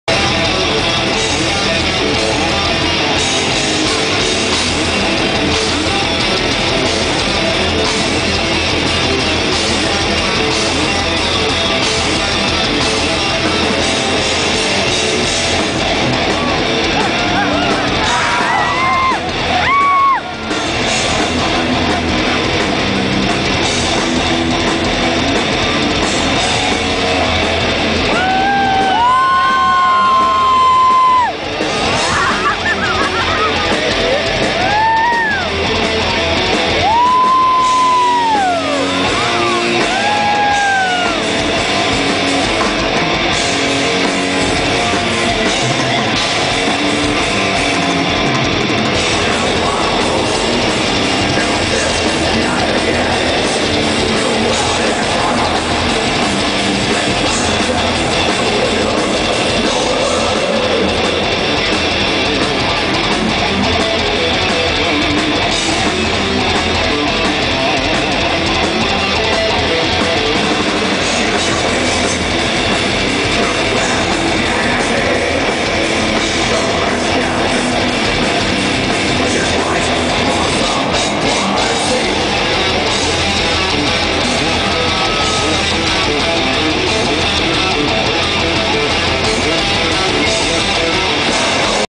CARCASS @ VFG Guadalajara México 26 feb 09 (1)
CARCASS was the second band for the Iron Maiden opening at the VFG arena in Guadalajara, I must say that they were really good, the blast beats were great, keep rocking guys, so do you Michael Amott